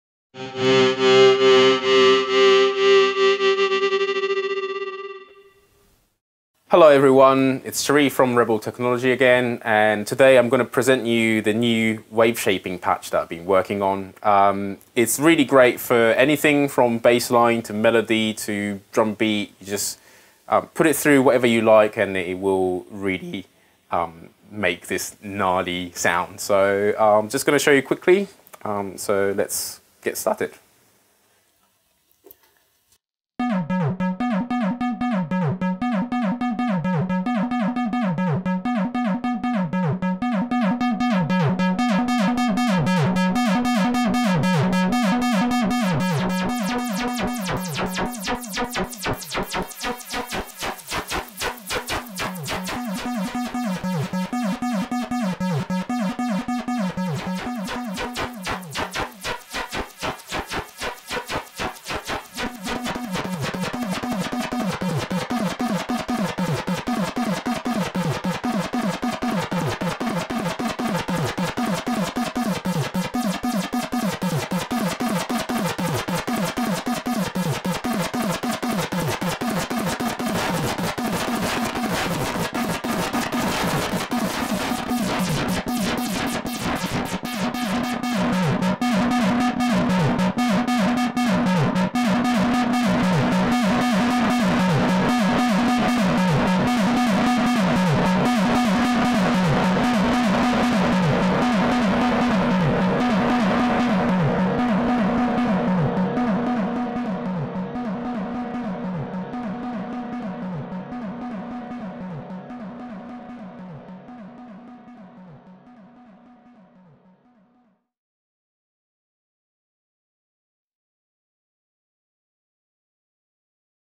OWL Modular ClingWrap Distortion
Cherif demonstrates the ClingWrap distortion, written in Max MSP's Gen for the OWL Modular and OWL pedal.

ClingWrap will let you morph between 4 different digital clipping algorithms to explore all that nasty digital harshness! Get it from hoxtonowl.com, and try it on your synth lines: